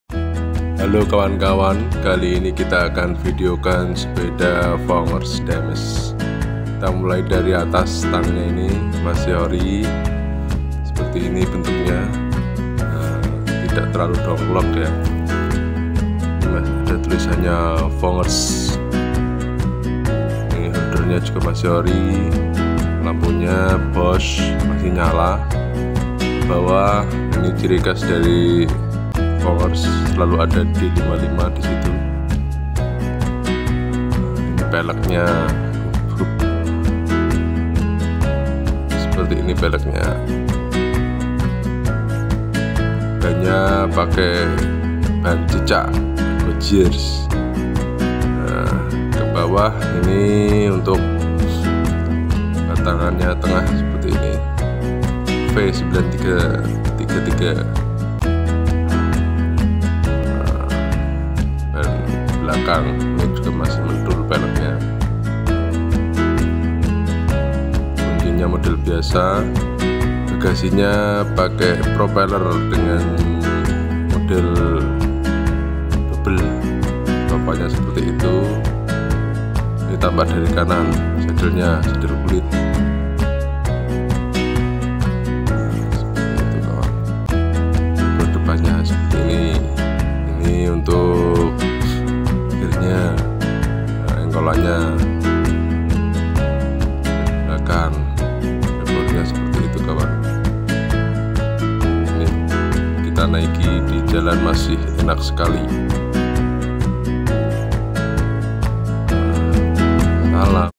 Fongers D55 sepeda Onthel Tua produksi tahun 1954. kondisi ori
sepeda fongers ini berasal dari kota groningen belanda.
kondisi masih cukup baik.
jika ada keminatan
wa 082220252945